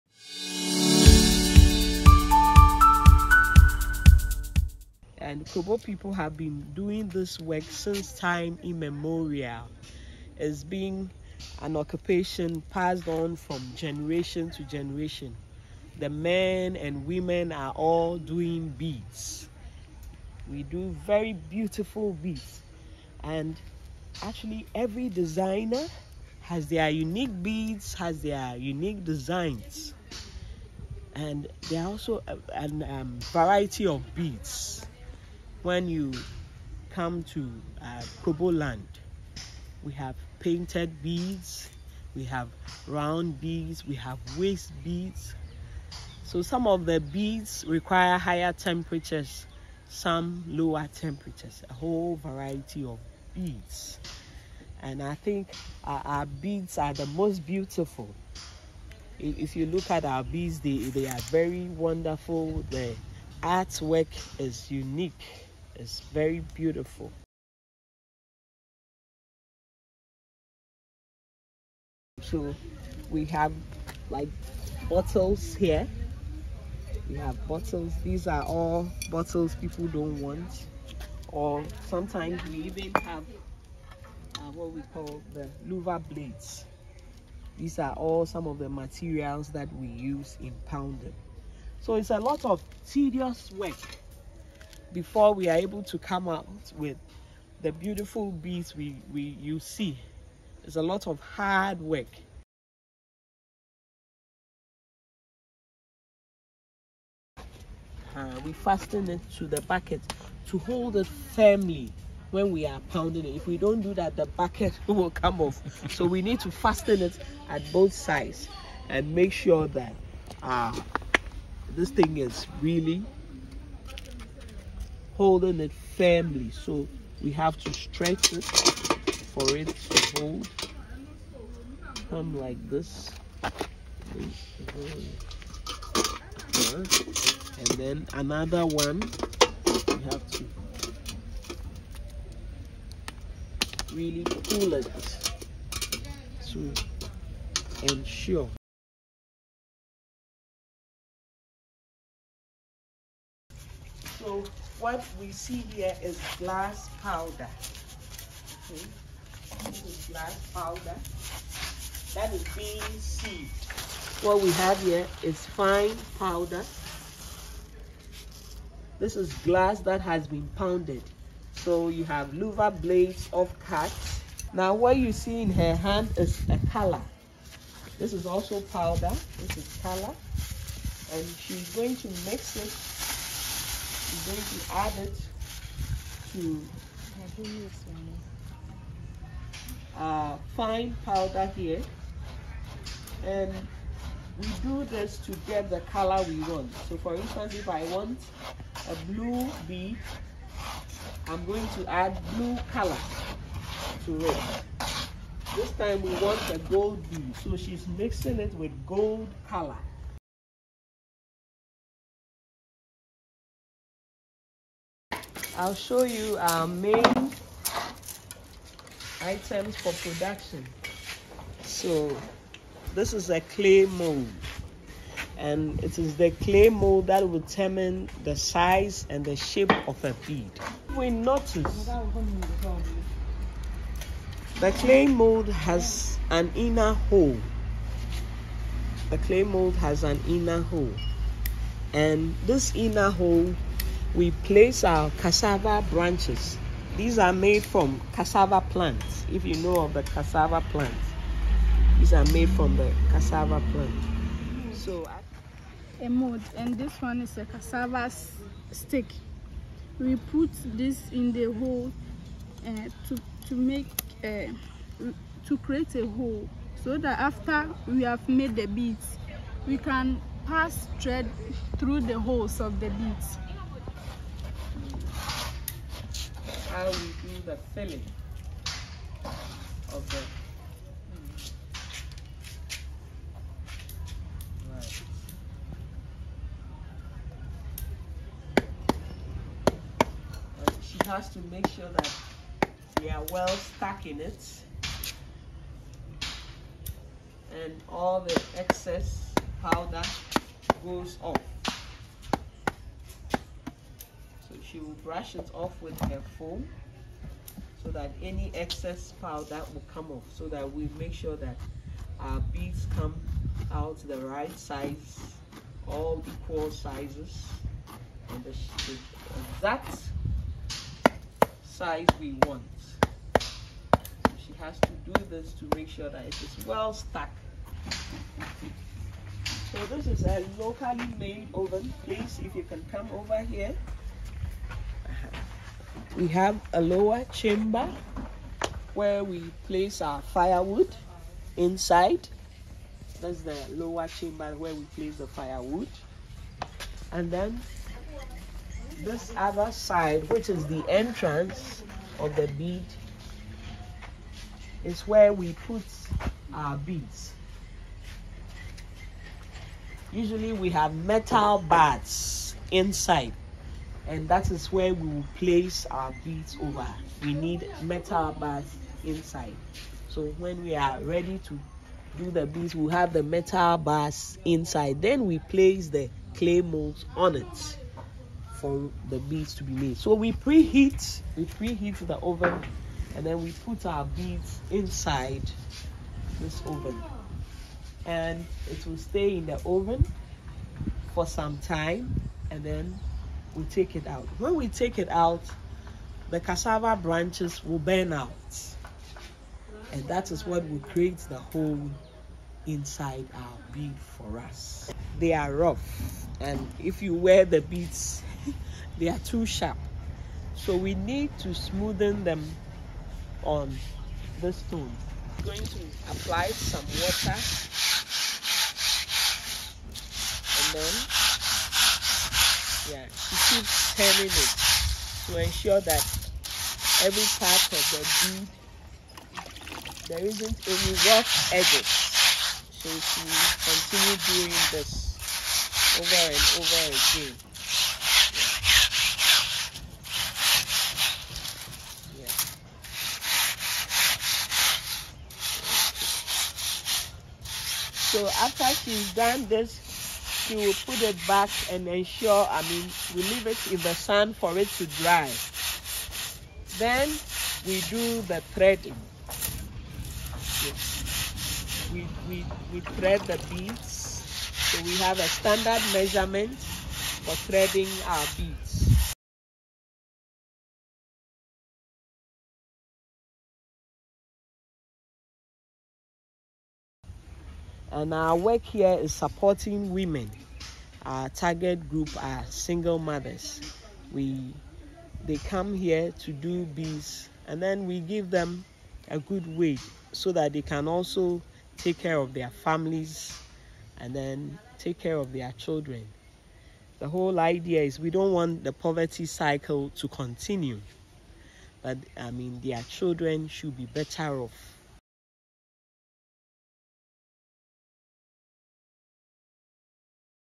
How African Krobo Glass Beads Are Made
Learn where we source our beads from and how they're made! House of Gems Believes in Fair Trade and Supporting the Local Women in Kroboland. We bring the Beauty of Ghanian Beads to you, while supporting the women who work hard to make them.